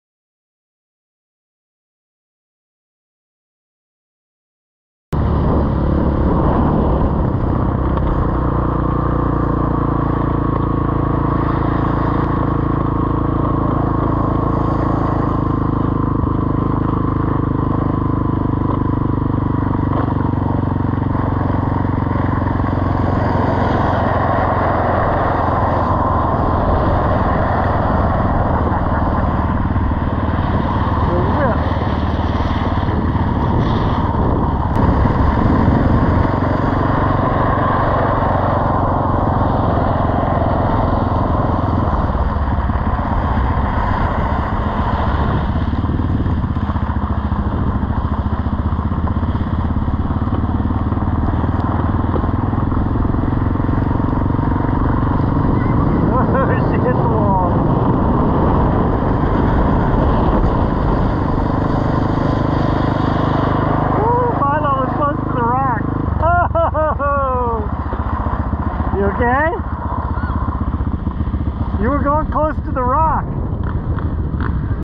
2 20 22 The Kelley's at Coldiron's pasture pond
The kids went for several rides at today's ice racing round.  Here's the beginning when it was still cold & the ice wasn't chewed up & slushy.